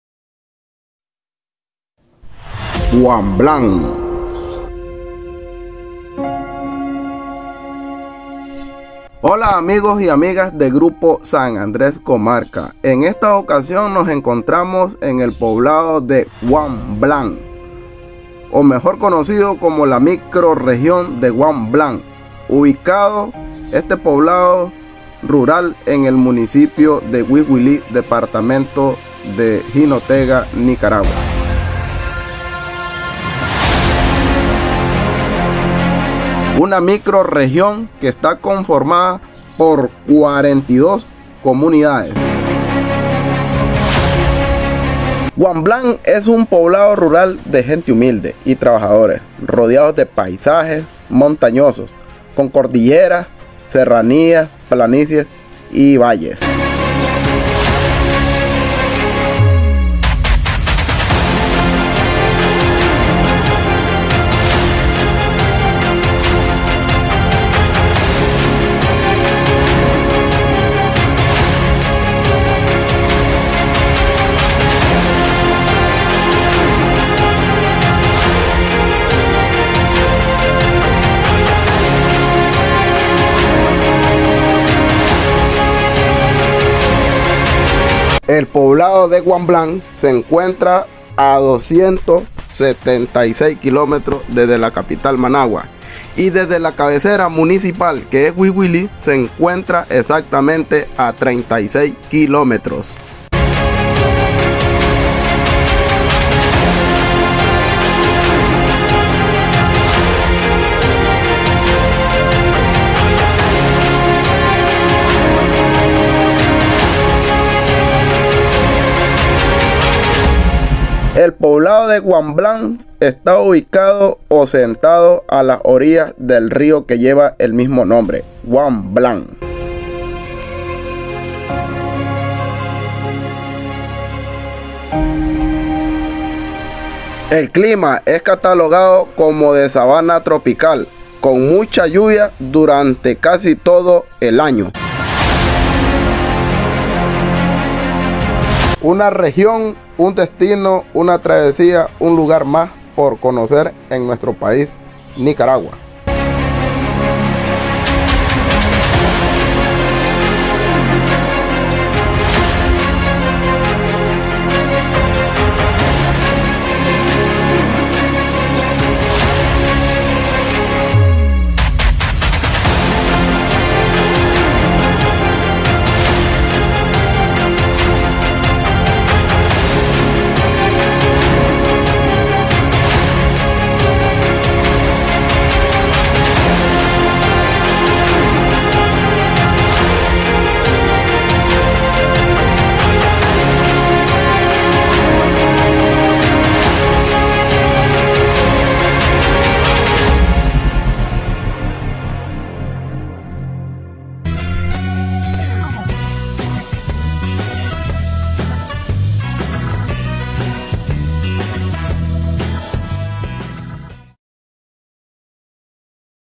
así es un pueblo rural del norte de Nicaragua - WAMBLAN
La micro región de wamblan cómo se le conoce es un poblado del departamento de jinotega Nicaragua llena de paisajes, de montañas y de gente laboriosa.